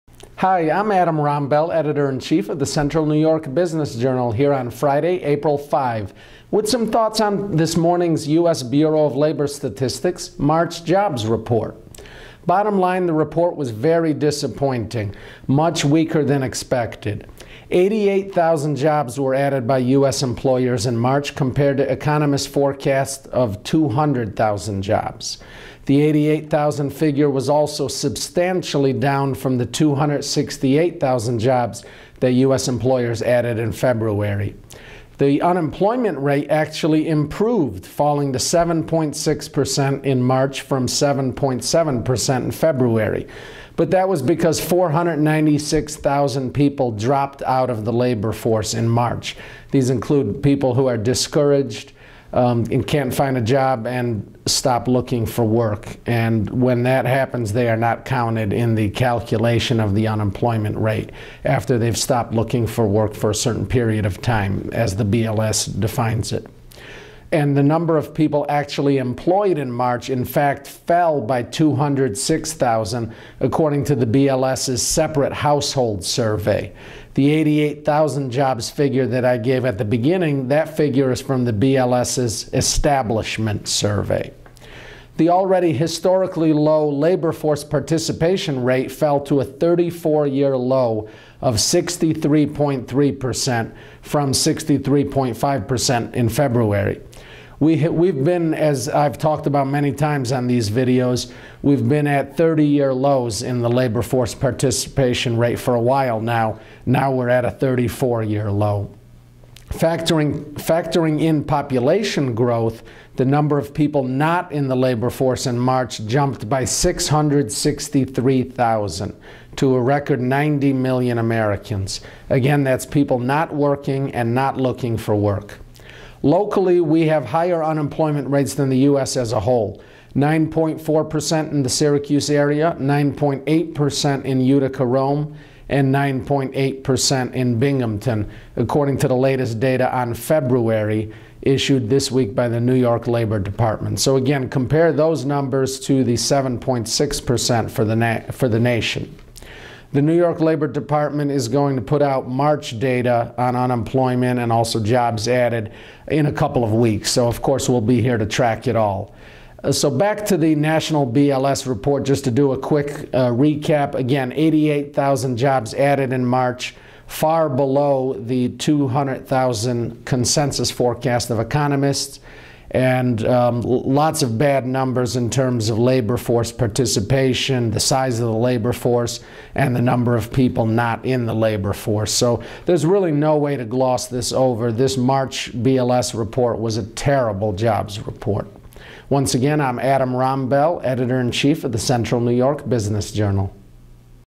Analyzing the March jobs report
Adam Rombel, editor-in-chief of The Central New York Business Journal breaks down the weaker than expected March jobs report. U.S. employers added just 88,000 jobs in March when economists had been expecting 200,000, according to the U.S. Labor Department. And, nearly 500,000 people dropped out of the labor force.